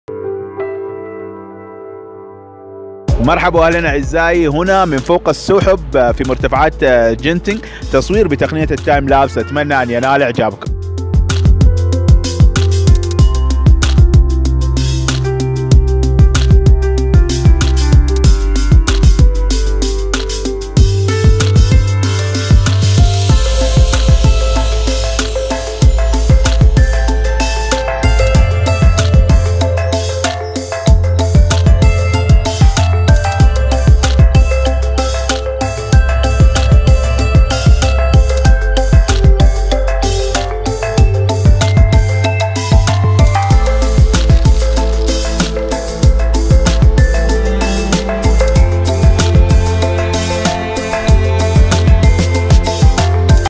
Timelapse Genting Highland Malaysia تايم لابس في مرتفعات جنتنغ ماليزيا
My Time-lapse video above the clouds in Genting Highland, Malaysia.
تصويري بتقنية التايم لابس فوق السحاب في مرتفعات جنتنغ في ماليزيا. طبيعة ساحرة اتمنى ان ينال الفيديو اعجابكم
تحياتي
محمد عثمان بلولة Mohamed Osman Baloola